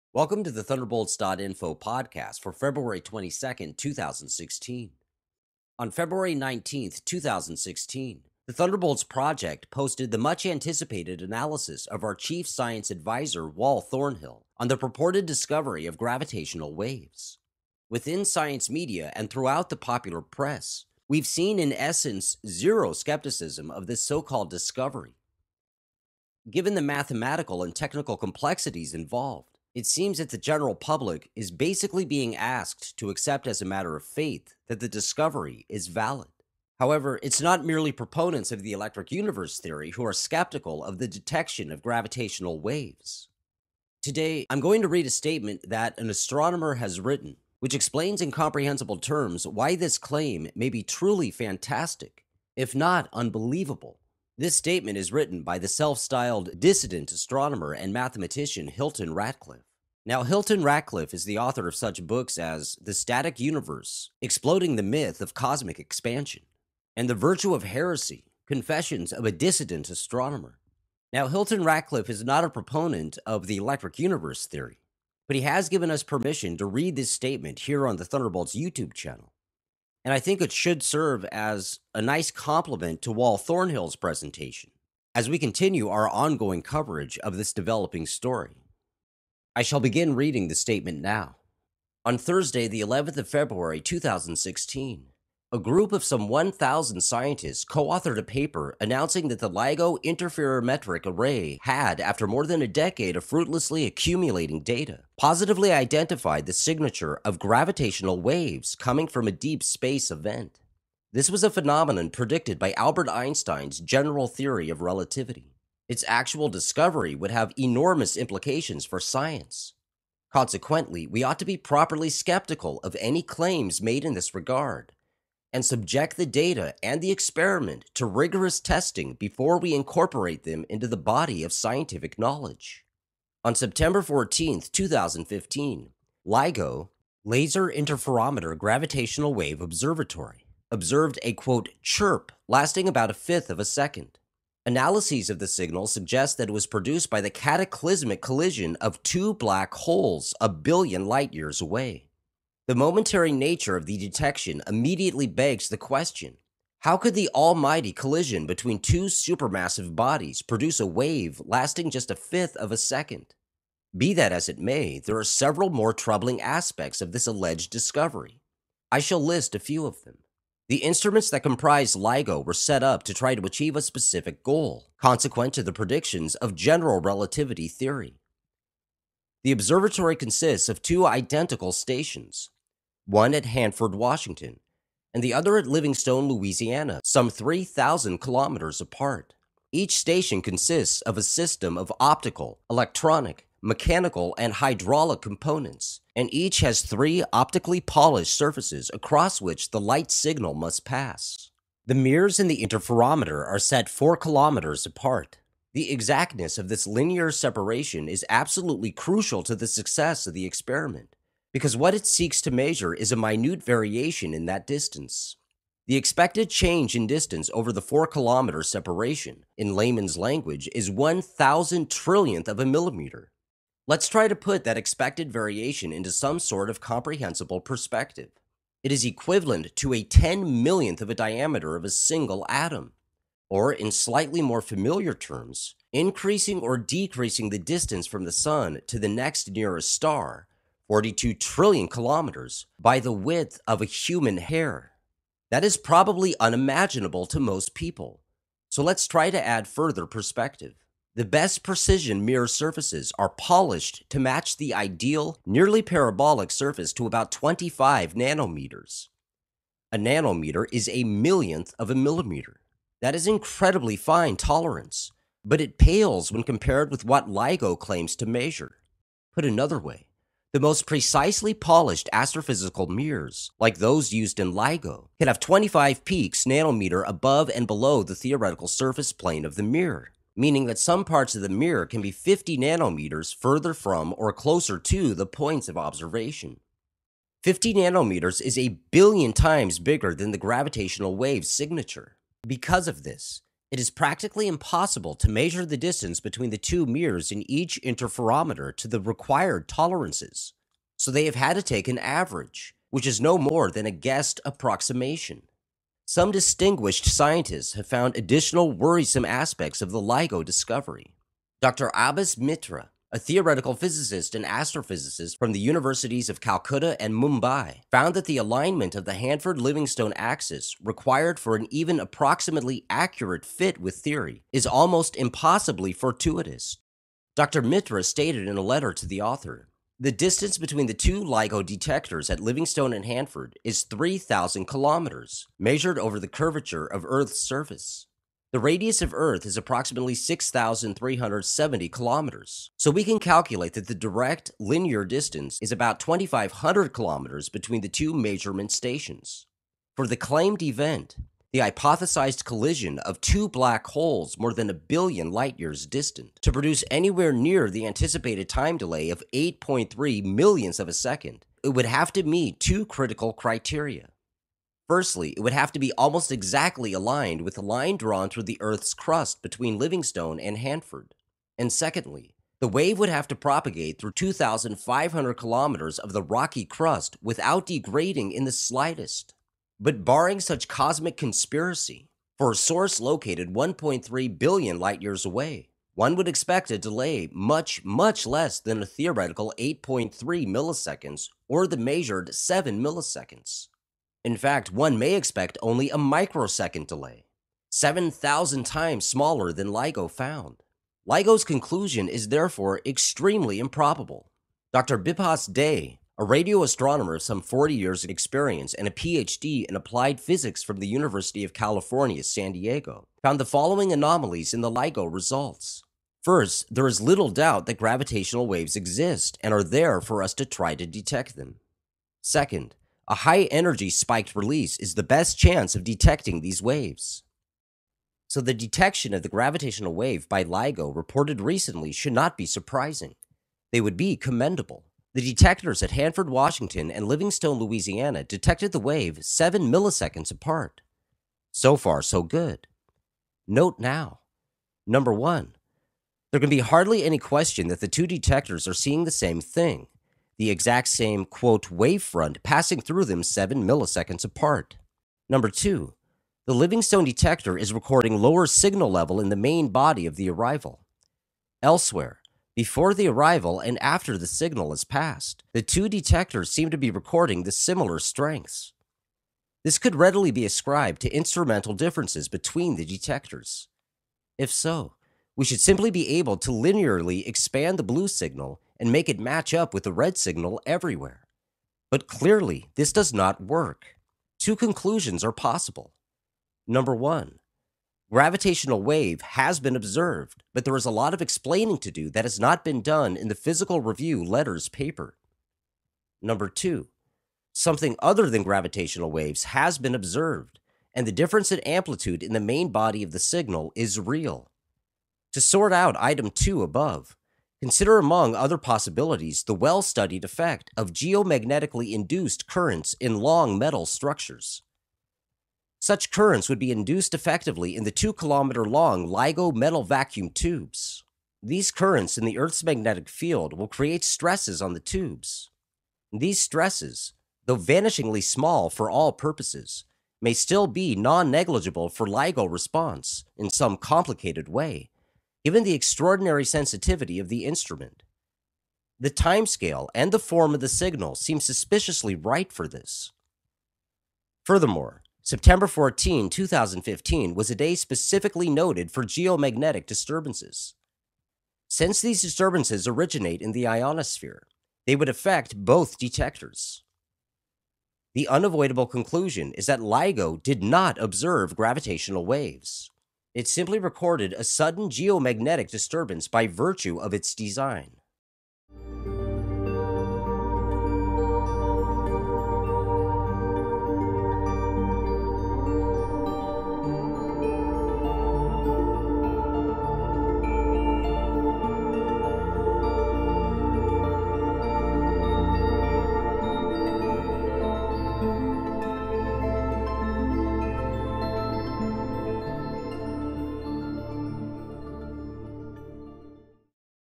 HILTON RATCLIFFE: "Discovery" of Gravitational Waves | Thunderbolts Podcast
On February 19, 2016, The Thunderbolts Project posted the much-anticipated analysis of its chief science advisor, Wal Thornhill, on the purported discovery of gravitational waves. Within science media and the popular press, there has been very little if any skepticism of this so-called discovery. Given the mathematical and technical complexities involved, it would seem that the general public is basically being asked to accept that the discovery is valid. However, it is not merely proponents of the Electric Universe theory who are skeptical of the detection of gravitational waves. Today, in this brief podcast, Michael Goodspeed reads a statement by astronomer Dr. Hilton Ratcliffe, which explains in comprehensible terms why this claim may be fallacious. Dr. Ratcliffe is the author of such books as "The Static Universe: Exploding the Myth of Cosmic Expansion" and "The Virtue of Heresy: Confessions of a Dissident Astronomer."

The ideas expressed in videos presented on The Thunderbolts Project YouTube Channel do not necessarily express the views of T-Bolts Group Inc or The Thunderbolts Project(TM).